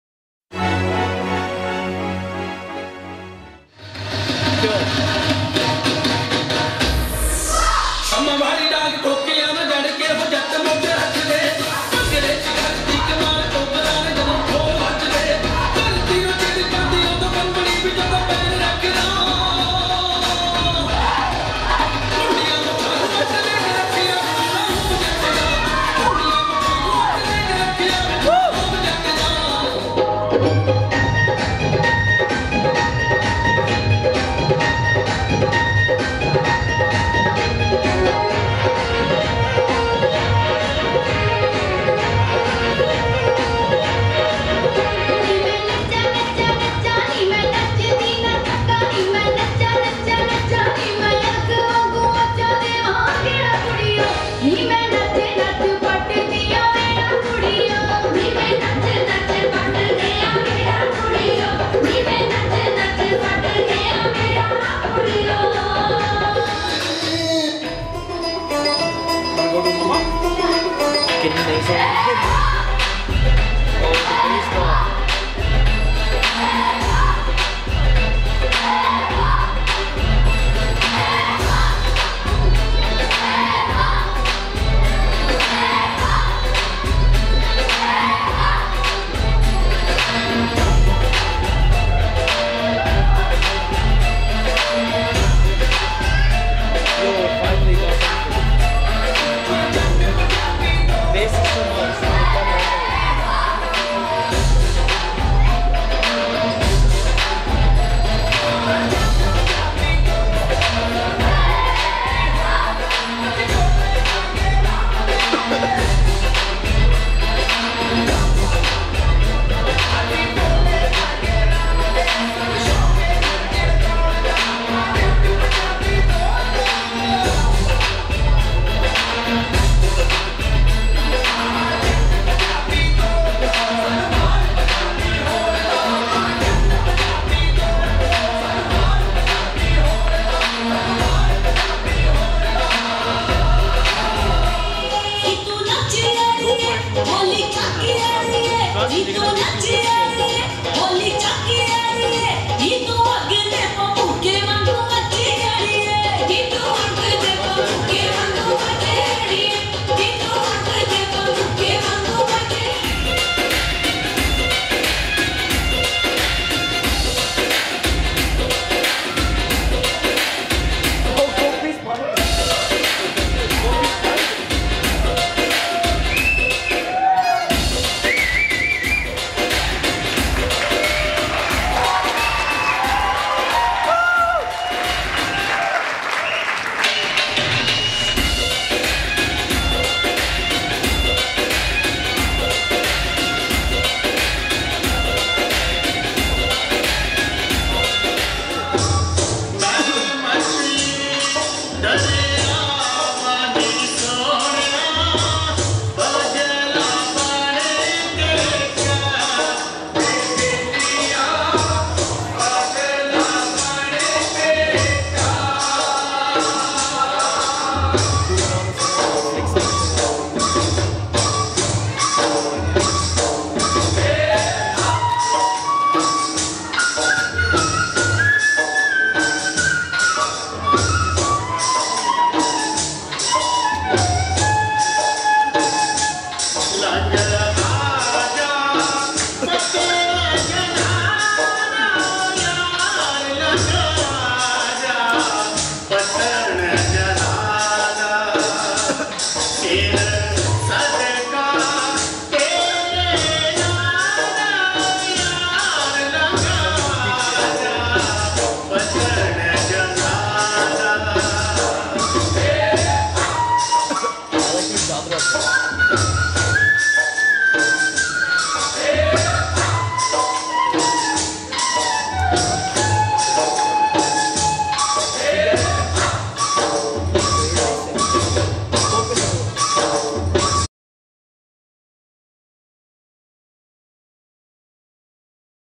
Colours of Punjab 2015
Royal Academy of Bhangra Annual Event.
We would like to thank all the Sponsors, Parents, teams & Supporters.
Special Thanks goes out to Shots of Scotch for supporting this event.